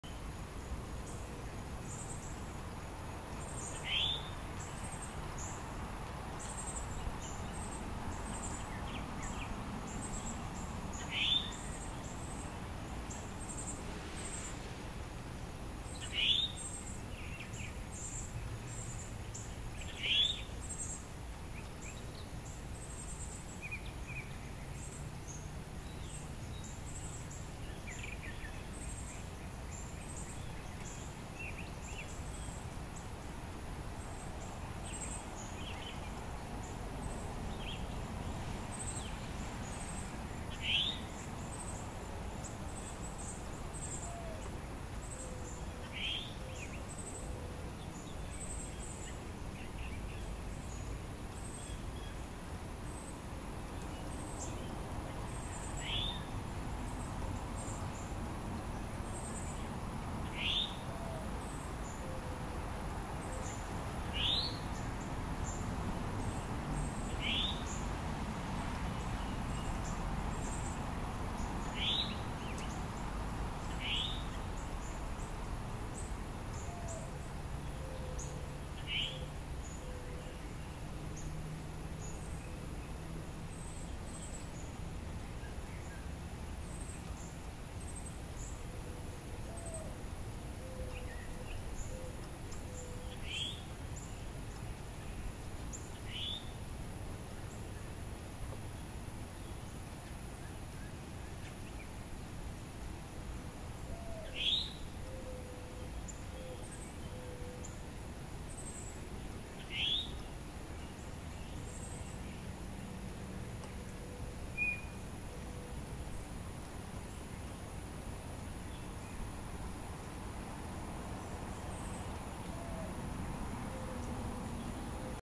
Male Cardinal Feeding a Juvenile
Filmed in Diamondhead, Mississippi in July 2016.